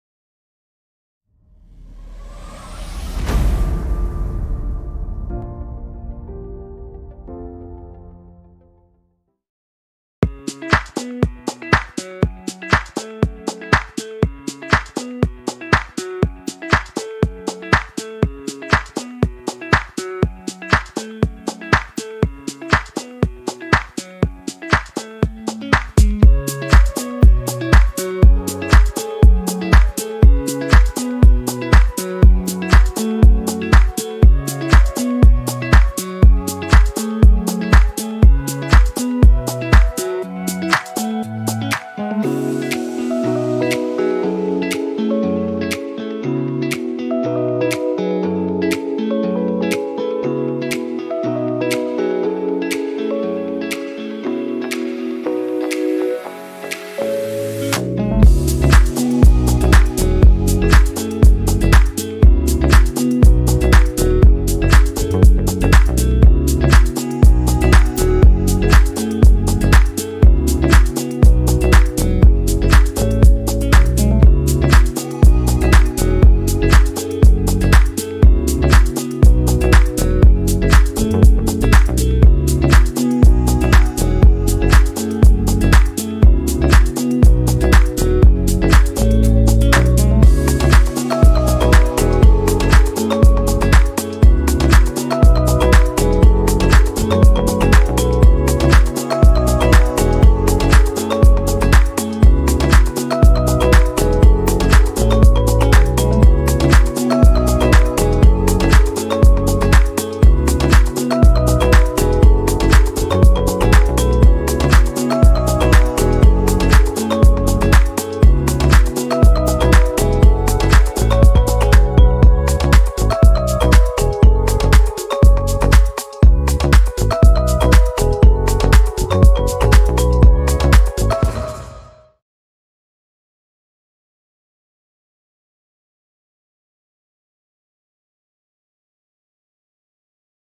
[ COVERING - PEELING ] : Suppression du covering liquide sur Hyundai Veloster
Nous réalisons des prestations de lavage à la main et de la personnalisation sur véhicules.

Le covering permet de changer la couleur de votre véhicule ou accessoire tout en gardant la possibilité de retrouver l'apparence originale.
Le detailing utilise des produits professionnel de nettoyage afin de retrouver l'apparence neuve de votre véhicule